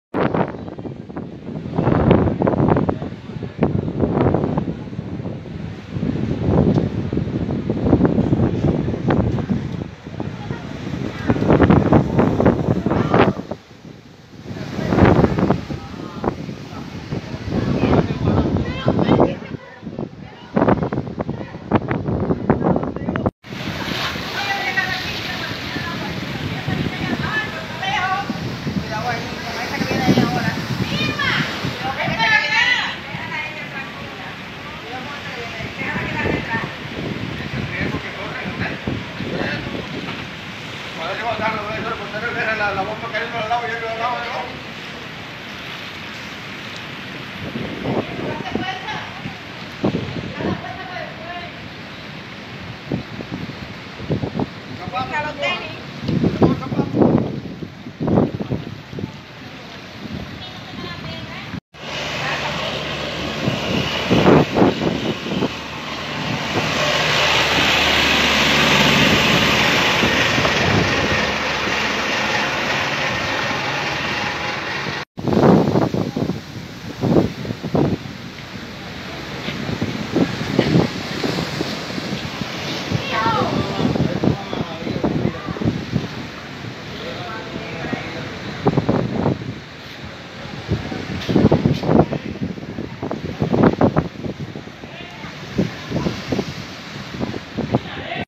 Reportan daños materiales por inundación en litoral de Baracoa
Los vecinos de Baracoa, Guantánamo, reportaron este jueves daños materiales por inundaciones costeras.

"Inundaciones costeras se producen hace varias horas el malecón de Baracoa y hay daños en viviendas situadas en esa zona del litoral. Los efectos del agua se notan, por ejemplo, en el llamado edificio de los médicos, donde fueron afectados algunas hogares", informó la emisora oficialista Radio Baracoa.

"Vecinos del área acudieron a socorrer a los afectados y pusieron a resguardo refrigeradores, televisores y otros equipos que trasladaron a casas cercanas. Agentes del orden público velan porque no se cometan indisciplinas en el área de los hechos".

La periodista oficialista Mirna Rodríguez Zúñiga compartió imágenes de los apartamentos afectados en Baracoa por inundaciones en el malecón.

Cubita NOW

Videos: Mirna Rodríguez Zúñiga.